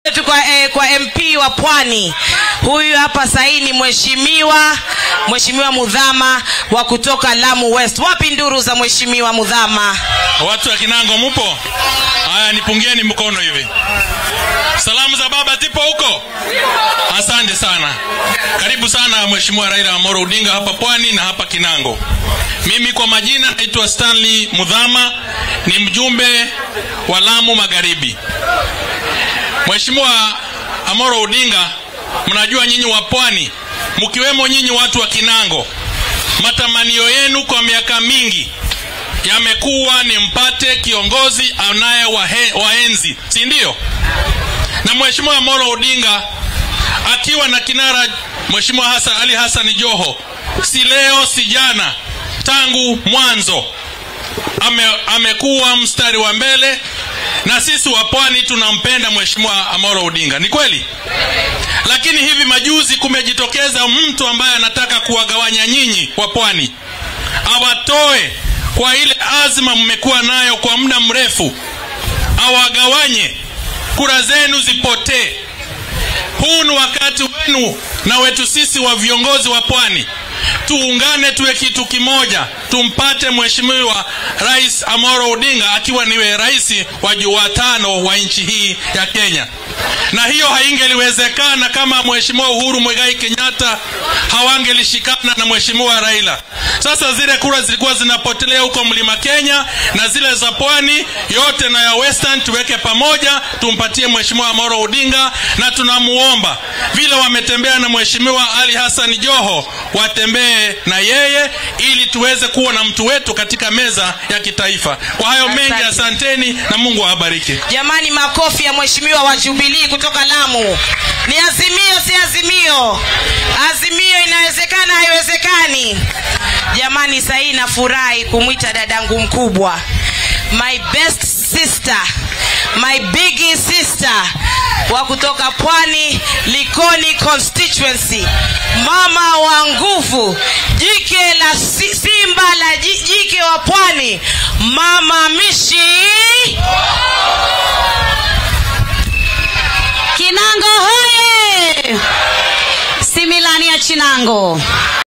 LAMU WEST MP STANLEY MUTHAMA HAILS RAILA ODINGA IN KWALE!!
RAILA ODINGA & HIS AZIMIO BRIGADE'S RALLY IN KINANGO, KWALE COUNTY!!